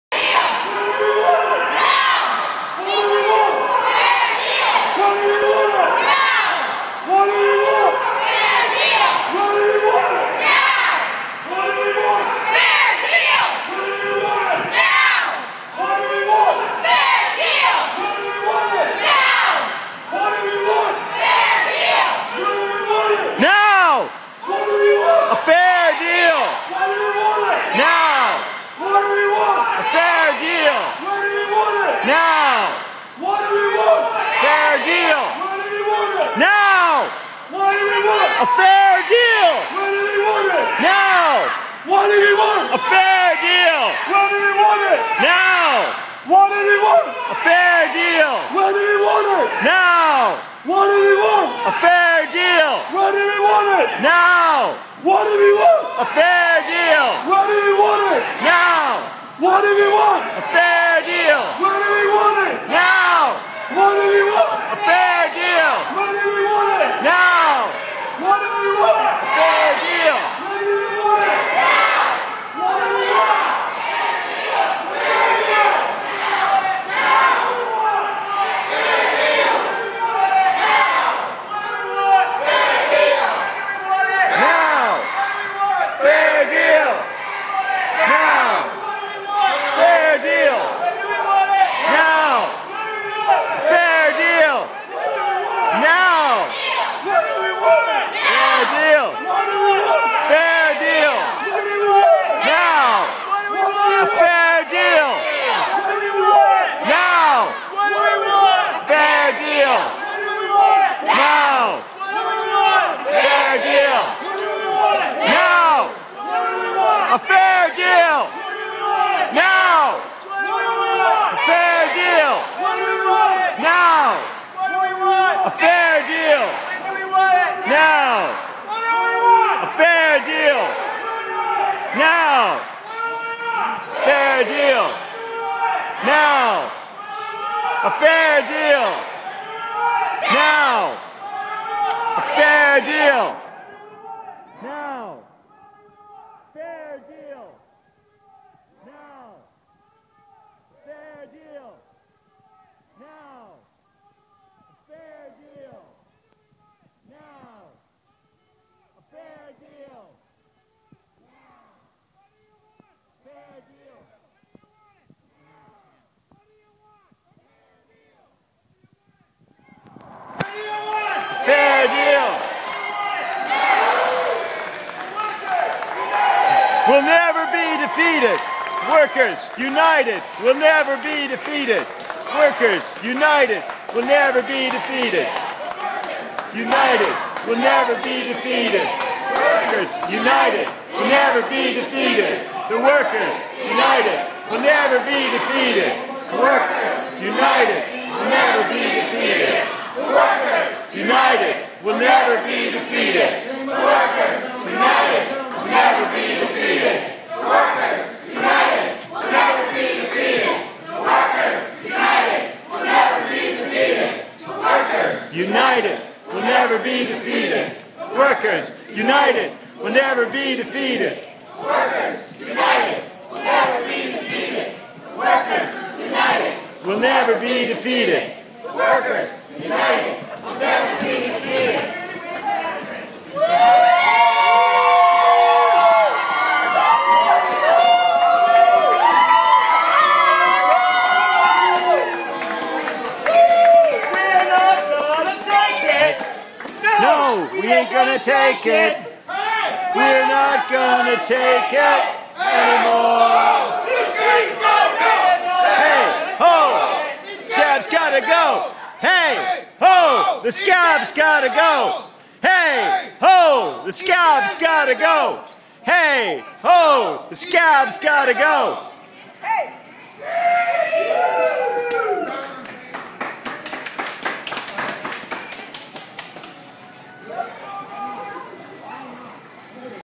Striking CAW members demonstrate inside Halifax Airport against Air Canada (1 of 2)
Members of the Canadian Auto Workers Union (CAW) striking against Air Canada's major concessionary demands on their existing pension plan take action inside the Halifax Stanfield International Airport on Wednesday June 15th in an effort to demonstrate their anger at both Air Canada's refusal to bargain in good faith as well as Tory Labour Minister Lisa Raitt's threats to remove their right to strike.